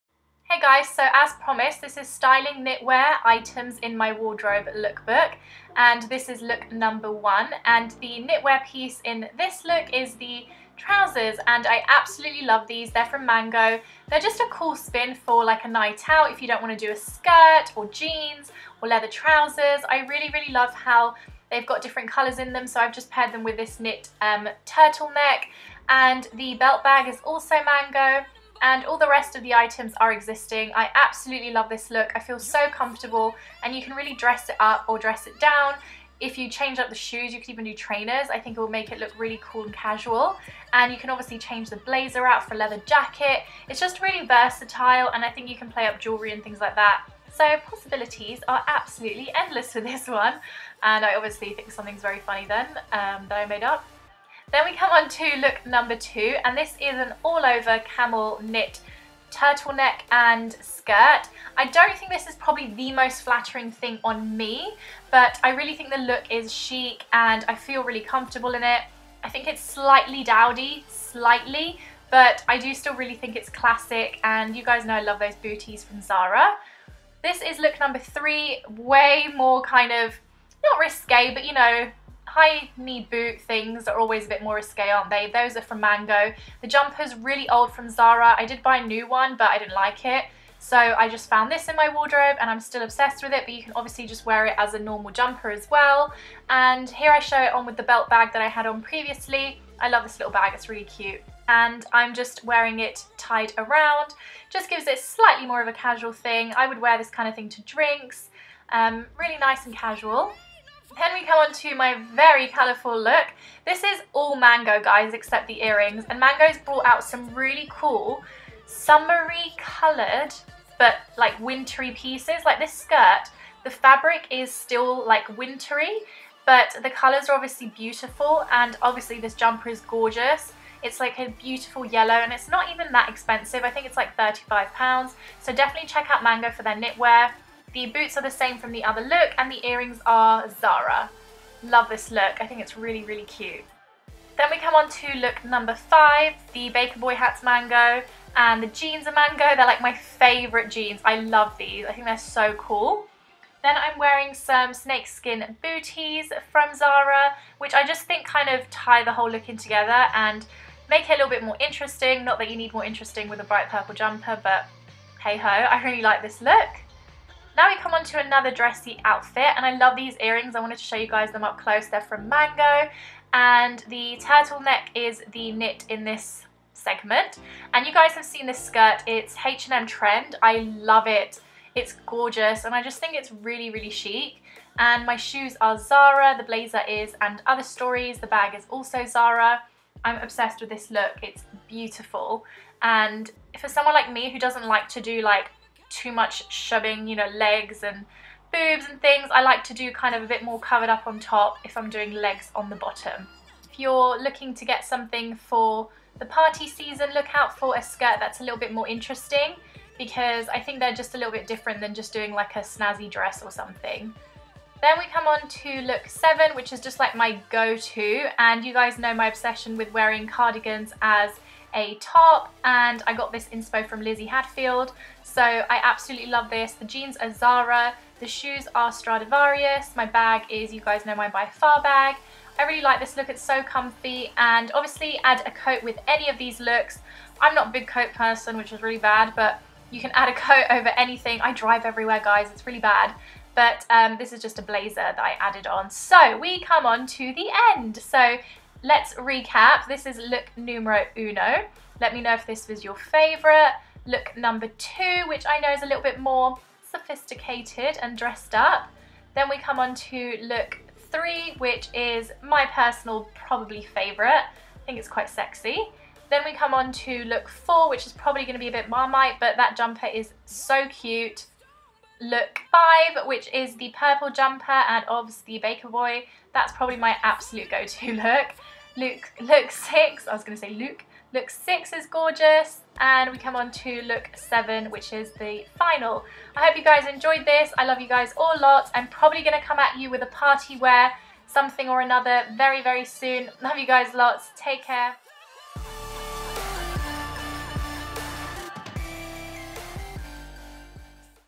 STYLING KNITWEAR IN MY WARDROBE!!!
Hello everyone :) As promised here is my styling knitwear items in my wardrobe video and I hope you all enjoy it! I think sometimes it’s hard to steer away from just wearing a knitted jumper so I hope you all find this helpful and get some ideas. I hope I got a good balance of smart/dressy and casual knits and most of them are from Zara, Mango and also &Otherstories!!! Let me know which is your favourite look down below 1,2,3,4,5,6 or 7???

TO BOOK IN WITH FI.CLINIC SEE BELOW
*we're based in Euston central London

— ITEMS SHOWN —

LOOK 1 //

Shoes: Zara

LOOK 2 //

Boots old Zara

Bag: Zara

LOOK 3 //

LOOK 4 //

Earrings: Zara

LOOK 5 //

By far RACHEL bag (cream colour is currently sold out but the others are lovely too!)

Snakeskin Boots: Zara

LOOK 6 //

Fringe skirt (the actual one is sold out boooo check stores)

Shoes: Zara

Bag: Zara

LOOK 7 //